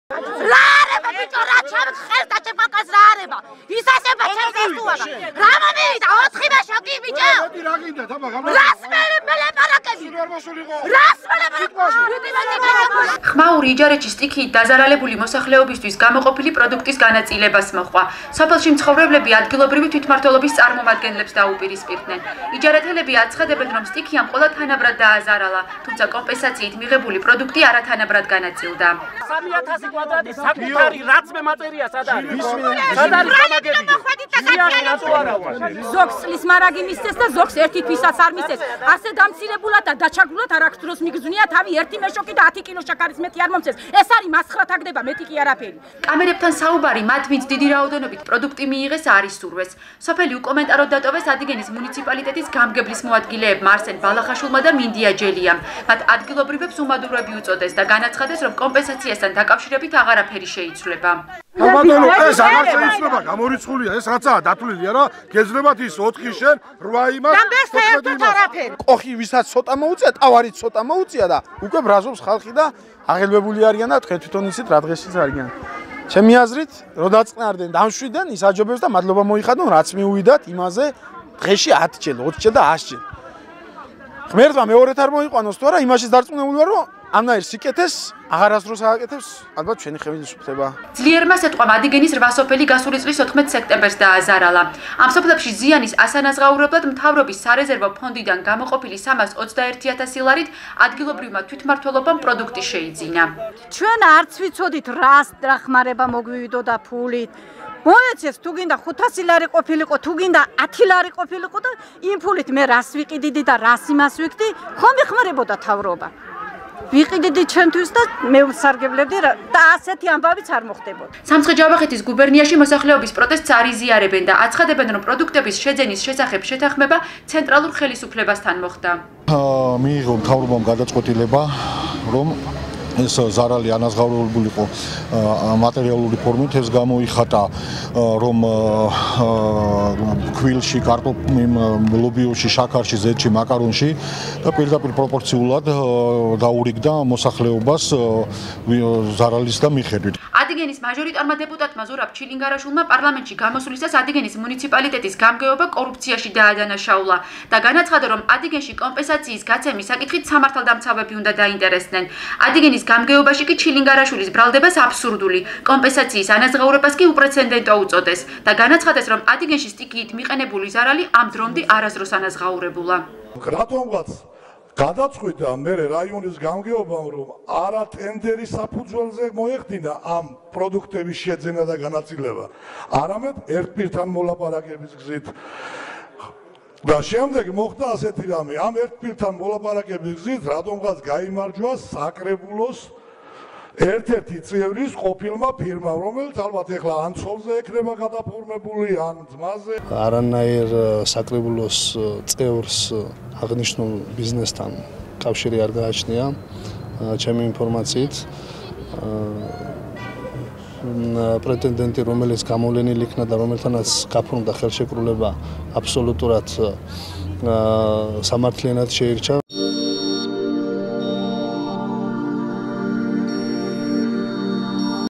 sknews.ge. უკმაყოფილება იჯარეთში
იჯარეთში სტიქიით დაზარალებული მოსახლეობისთვის გამოყოფილი პროდუქტის განაწილებას ხმაური მოჰყვა. სოფელში მცხოვრებლები ადგილობრივი თვითმმართველობის წარმომადგენლებს დაუპირისპირდნენ. იჯარეთელები აცხადებენ, რომ სტიქიამ ყველა თანაბრად დააზარალა, თუმცა კომპენსაციით მიღებული პროდუქტი არათანაბრად განაწილდა.